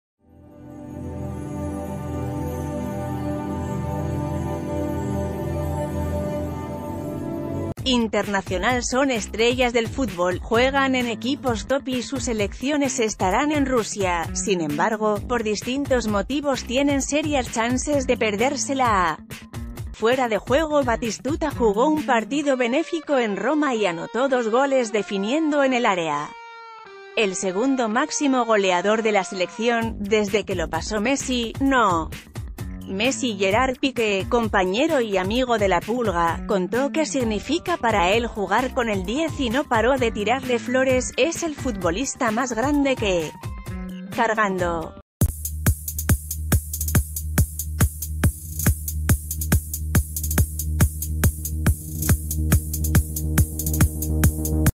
Noticias de última hora | Con este Cúper no, Cristiano
INTERNACIONAL Son estrellas del fútbol, juegan en equipos top y sus selecciones estarán en Rusia. Sin embargo, por distintos motivos tienen serias chances de perderse la ... FUERA DE JUEGO Batistuta jugó un partido benéfico en Roma y anotó dos goles definiendo en el área. El segundo máximo goleador de la Selección, desde que lo pasó Messi, no ... MESSI Gerard Piqué, compañero y amigo de La Pulga, contó qué significa para él jugar con el 10 y no paró de tirarle flores: "Es el futbolista más grand...